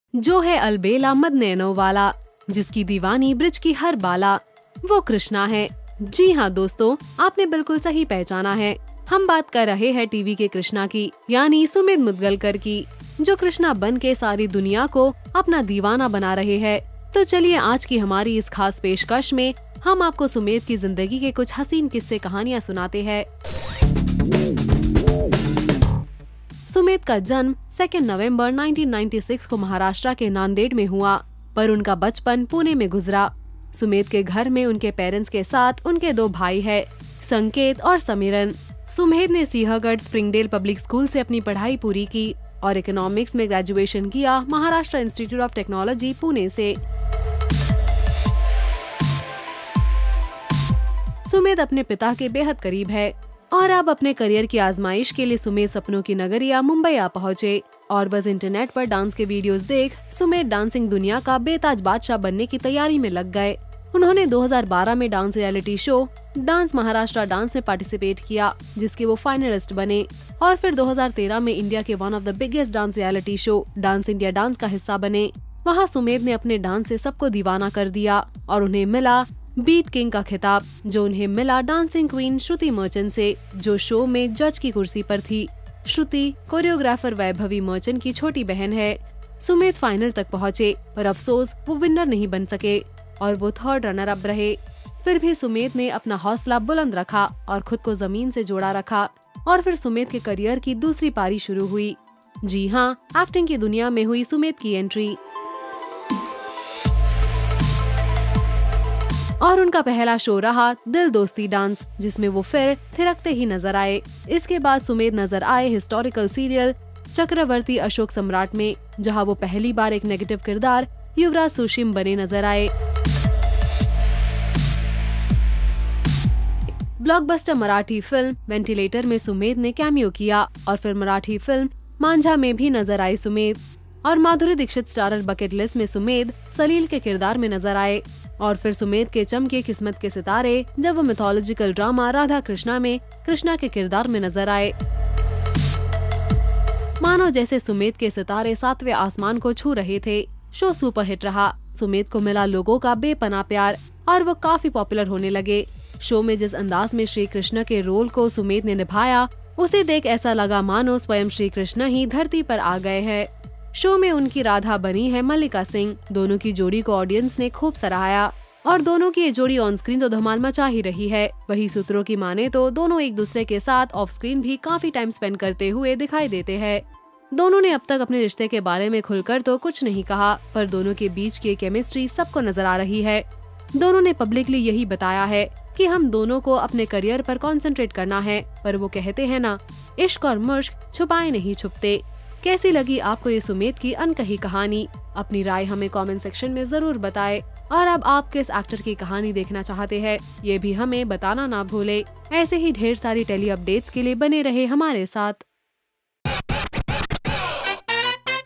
KAHANI SUMEDH KI | Life Story Of Sumedh Vasudev Mudgalkar | Biography | Dance India Dance
Watch Lifestory Of Sumedh Vasudev Mudgalkar where you will know about his childhood, struggle, dancing, acting, stardom and much more in our special segment Kahani Sumedh Vasudev Mudgalkar ki only on Telly Masala.